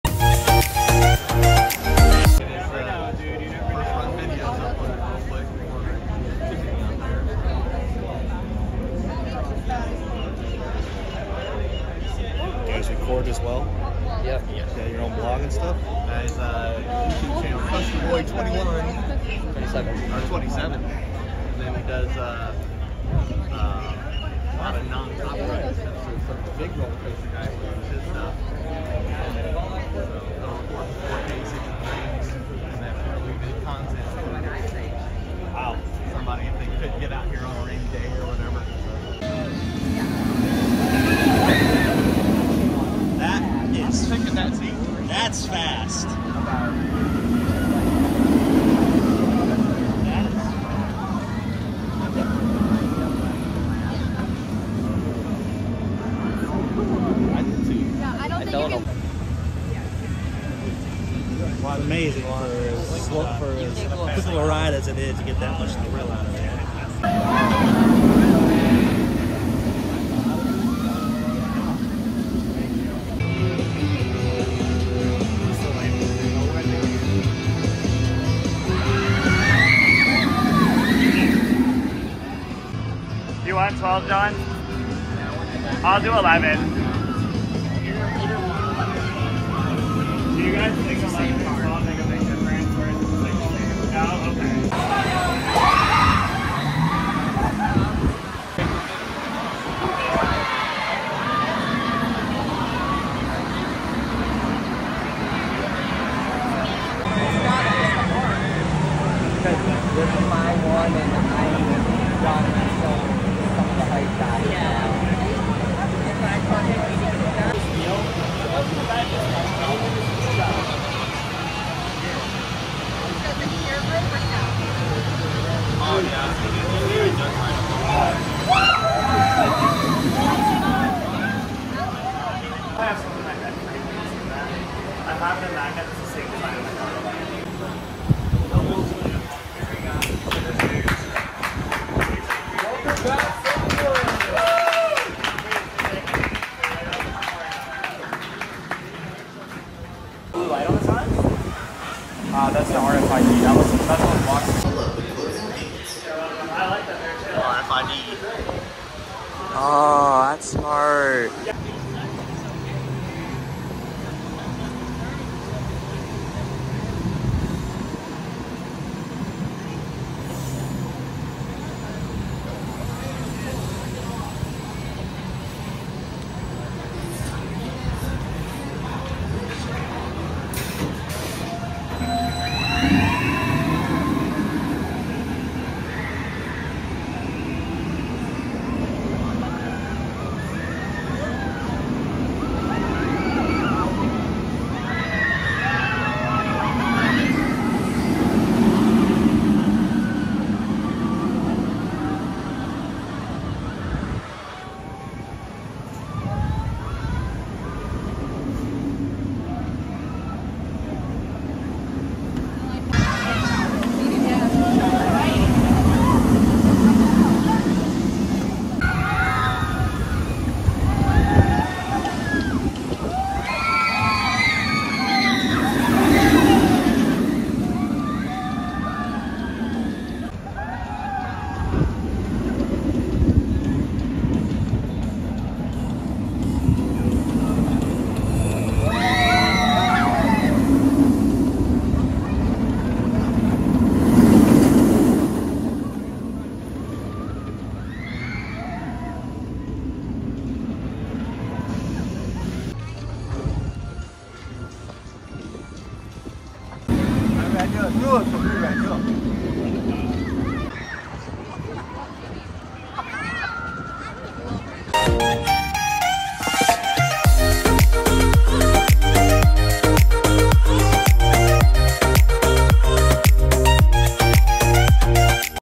Pipeline: The Surf Coaster Offride 4k 60fps 2023 Copyright Free SeaWorld Orlando
This is offride footage of Pipeline: The Surf Coaster. This is the brand new prototype surf coaster by B&M at SeaWorld Orlando. It is a new generation stand up coaster with seats that move up and down with the forces of the ride.